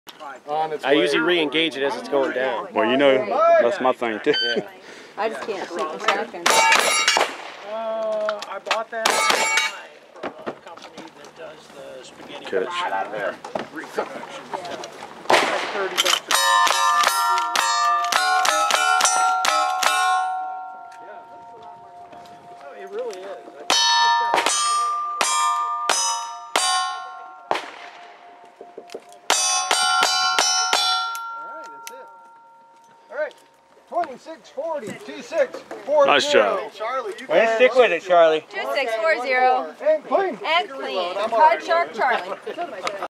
Stage 4 Card Shark Charlie Raiders 12 4 22
Roughshod Raiders December Monthly Match at Shoot GTR in Gainesville, FL on 12/4/2022.

Top 5 Overall:
1) Bucky Buckskin – RAD - 93.02
2) JP Law – RAD - 100.28
3) Suwannee Kid – COW - 116.07
4) Arcadia Outlaw – RAD - 117.17
5) Delta Glen – RAD - 120.94

Top 5 Ladies:
1) Midwest Hale - RAD
2) Tilly Two Spurs - LWRA
3) Annabelle Bransford - LSS
4) Madame Moog – L49
5) Sure Shot Shawanda – L49

Clean Shooters:
Lainey Mae, Annabelle Bransford, Kid Romeo, Marshall J.W. Stone, Toothpick, Shelleen, Riverview Rattler, Suwannee Kid, JP Law, & Bucky Buckskin

Dagnabbit:
Rowdy Bill, Rooster Ray, Tilly Two Spurs, Rocky Creek Shooter, Wabash Valley Slim, & Arcadia Outlaw